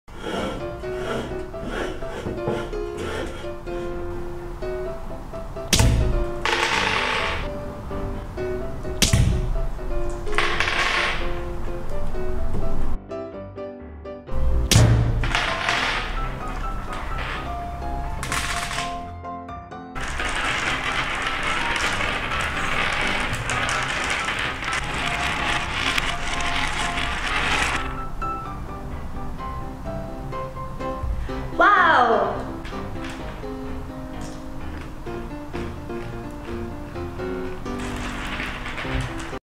Dora turns soda into candy
Dora turns soda into candy, funny challenge video.